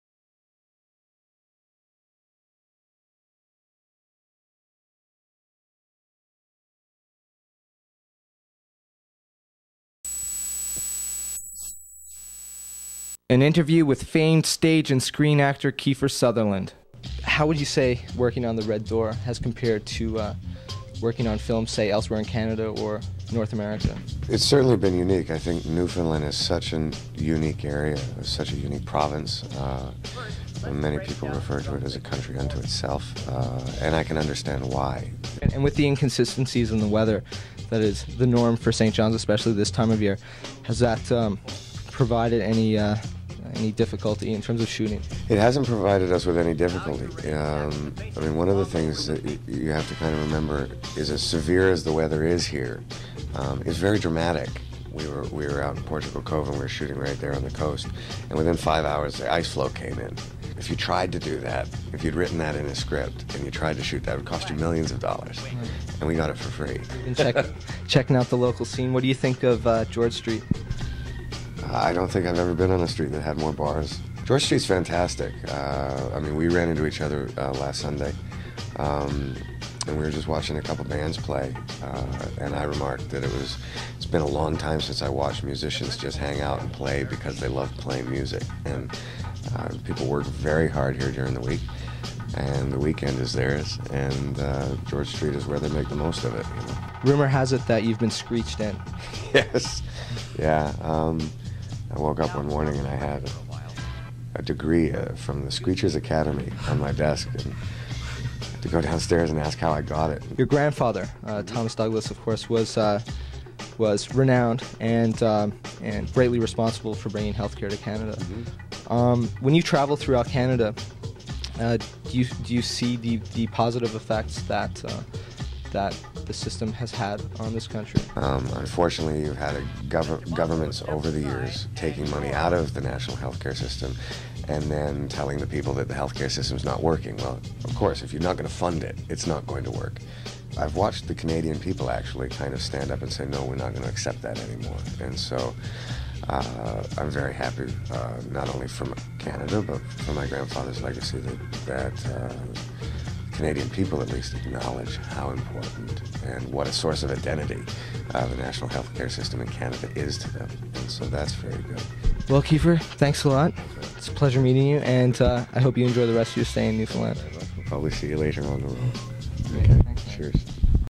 Anthony M. Tooton interviews Kiefer Sutherland in St. John's, Newfoundland, during the Sieved years.
One for the ages! A gentleman of the highest order - and a proud Canadian!